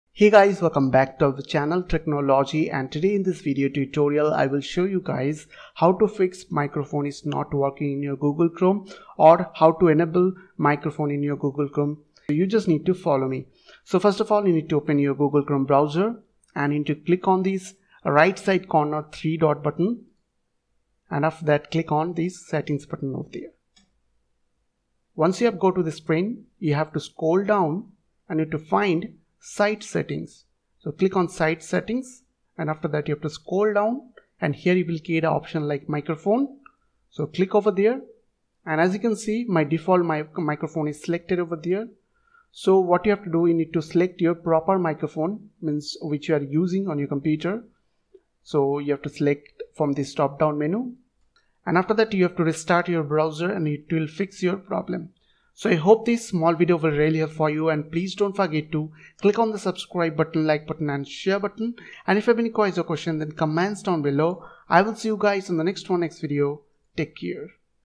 How To Fix Microphone not Working in Google Chrome
In this video tutorial, I will show you guys how to fix Microphone not working in your Google chrome.